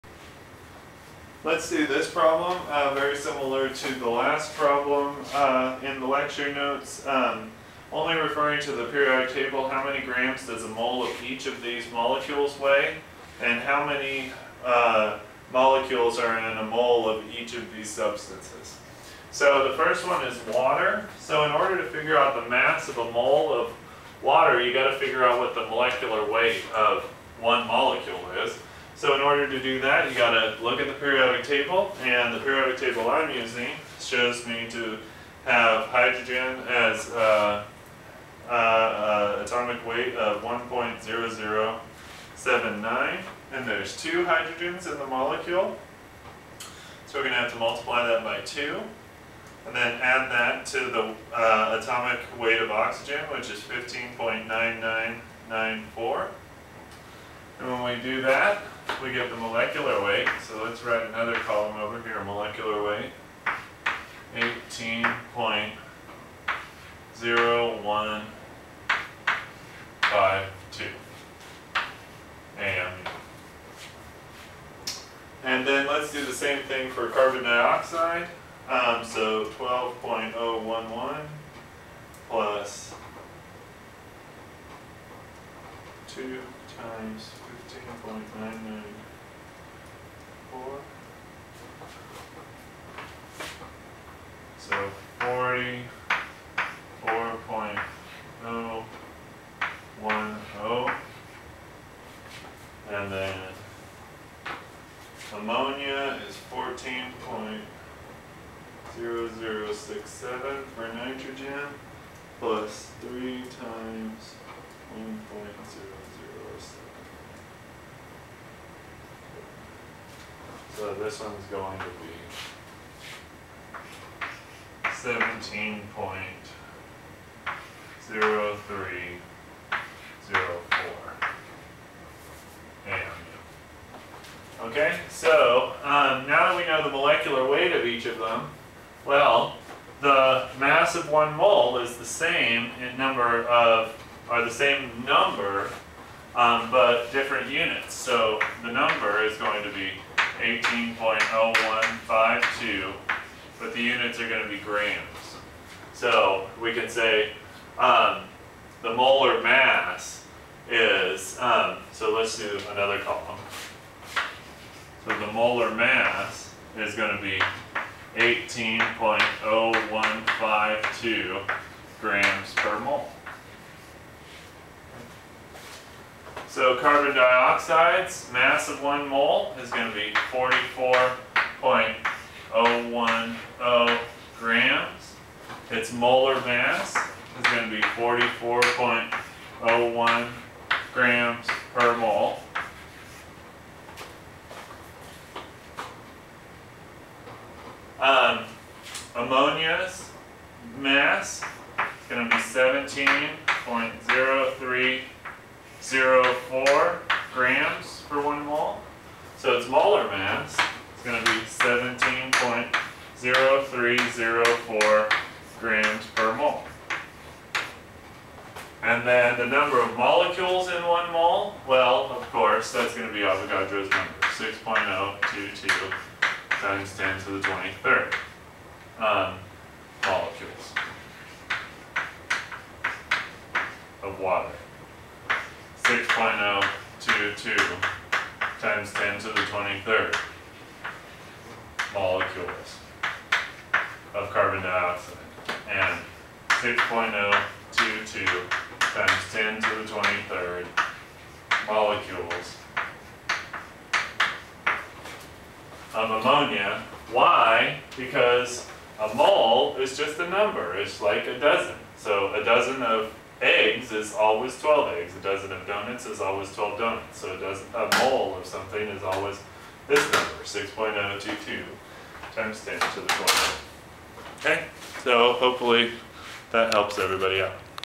Mole concept molecules 015
Only referring to the periodic table:
1) How many grams does a mole of each of these molecules weigh?

2) How many molecules are in a mole of each of these?

H2O CO2 NH3